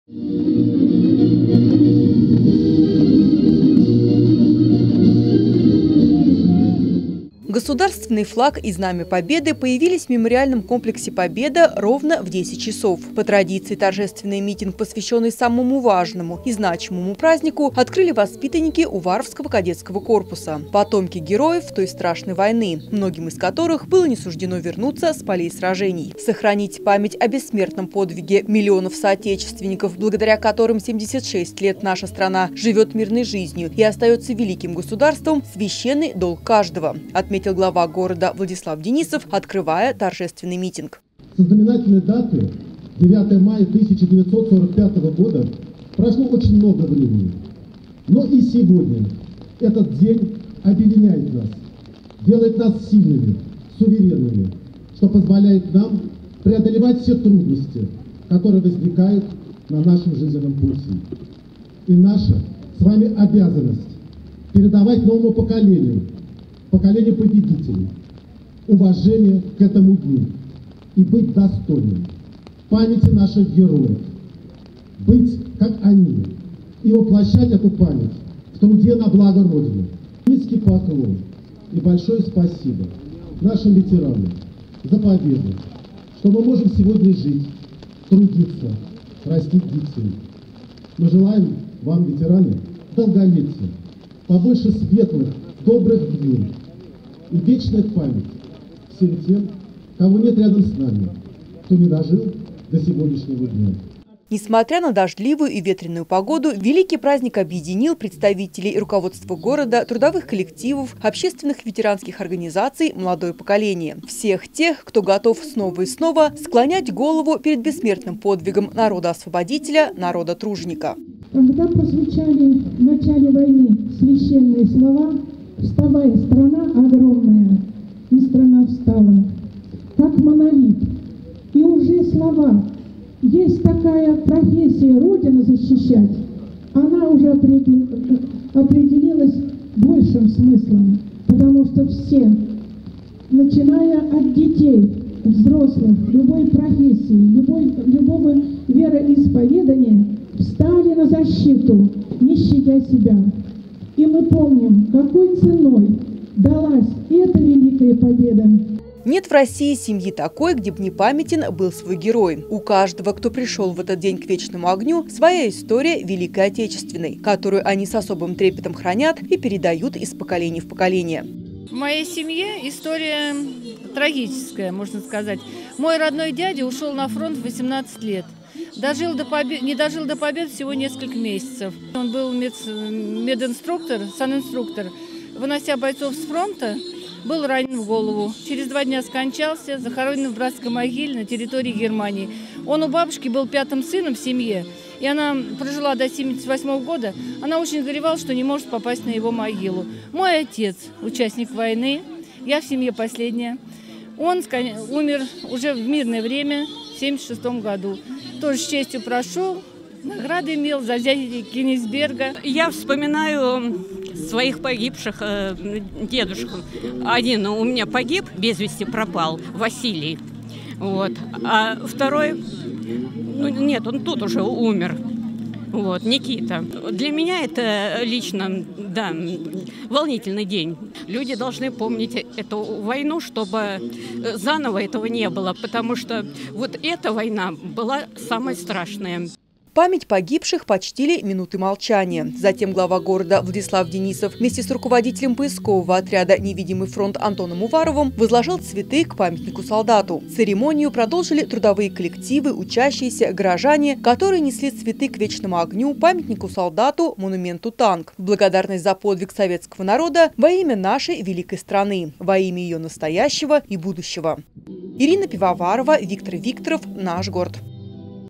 Самый важный и значимый
9-го мая уваровцы вместе со всей страной отметили 76-ю годовщину Победы в Великой Отечественной войне. В этот светлый день потомки героев собрались на торжественный митинг у Вечного огня, чтобы почтить память павших, чествовать тех, кто дожил до наших дней, пройдя все испытания войной, и напомнить молодым о том, какой ценой достались сегодняшние мирные дни.